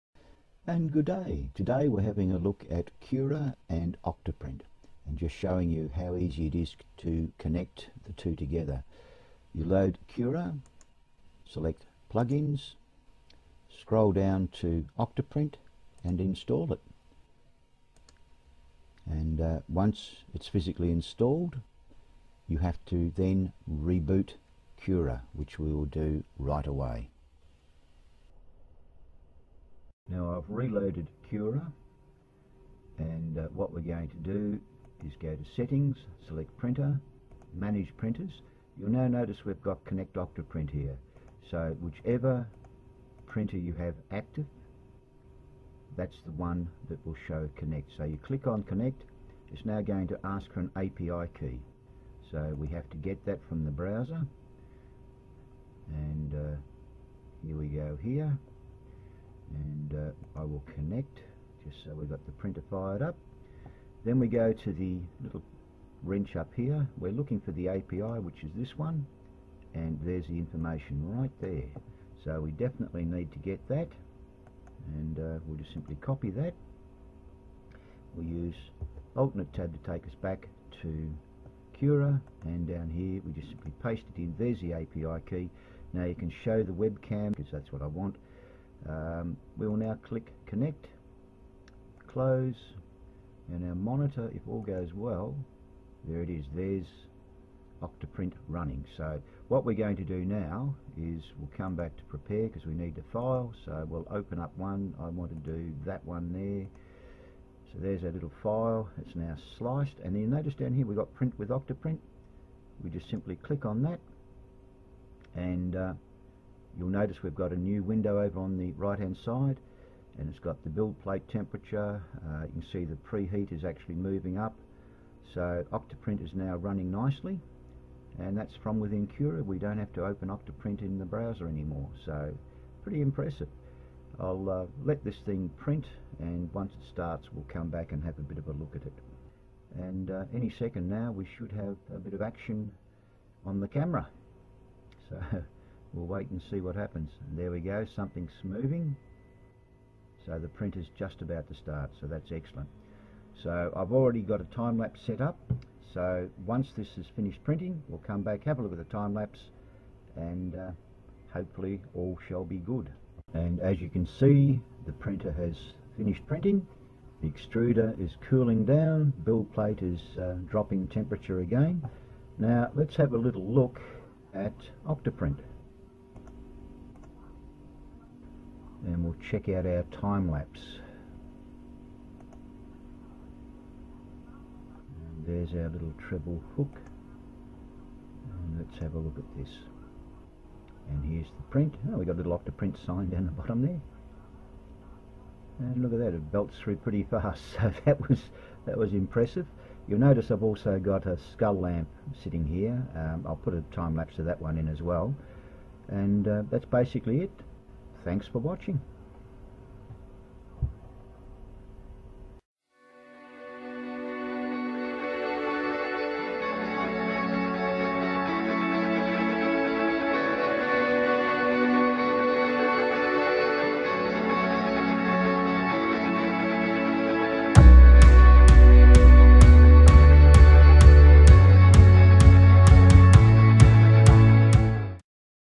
📌 Setting Up Cura 3.2.1 Octoprint Plugin using the Octoprint API Key for a 3D Printer
Setup Cura 3.2.1 using the Octoprint plugin and API Key on a Raspberry Pi 3 WiFi.

I also have a logitech c270 webcam connected to the Raspberry Pi 3, so I can see the 3D printer in action, and can remotely stop the print using Octoprint, if I see something going wrong.

There are also multiple apps for your mobile smart phone to monitor whats happening.